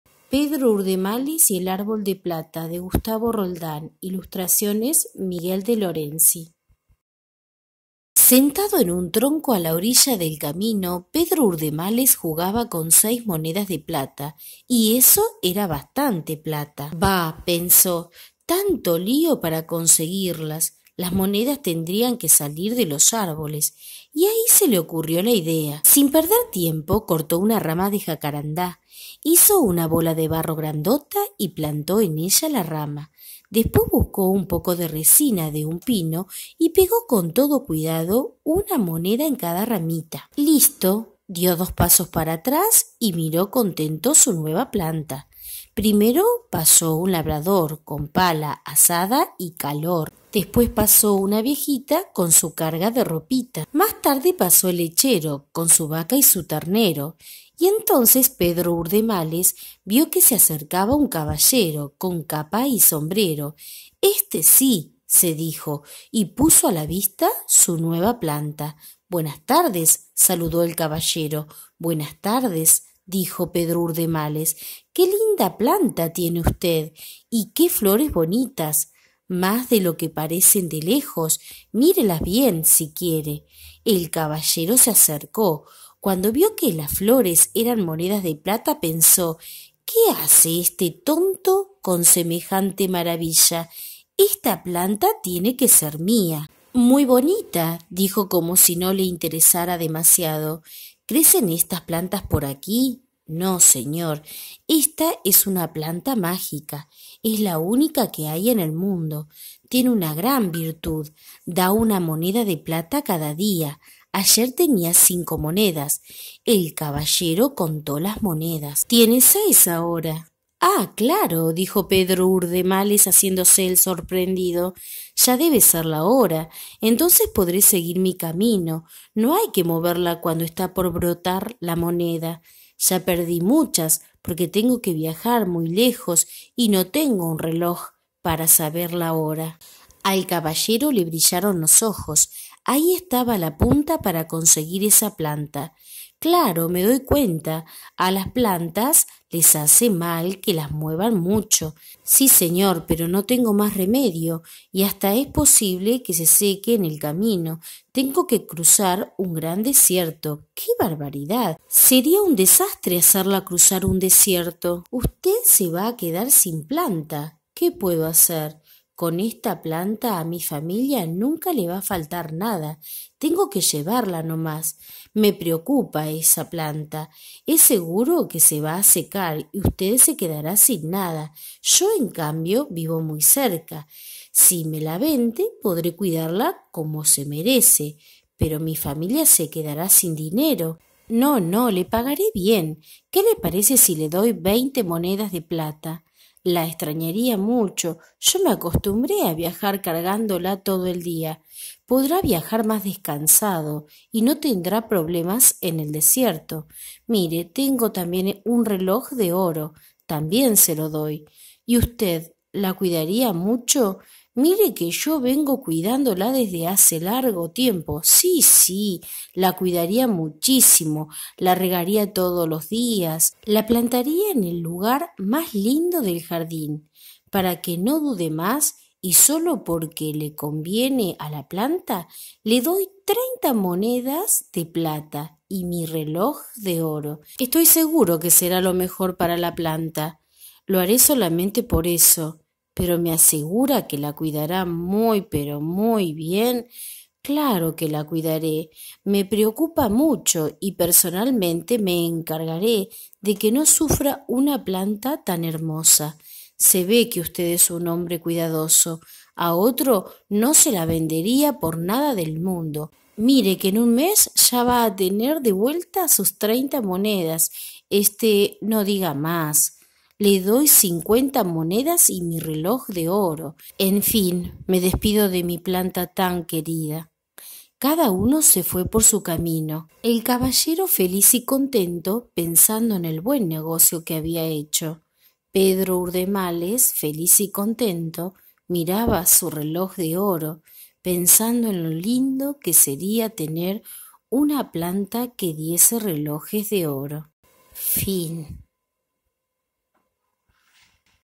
PEDRO URDEMALES Y EL ÁRBOL DE PLATA- GUSTAVO ROLDÁN
Versión libre de cuento popular.
Ilustraciones: Miguel de Lorenzi.
Cuentos del Pajarito Remendado.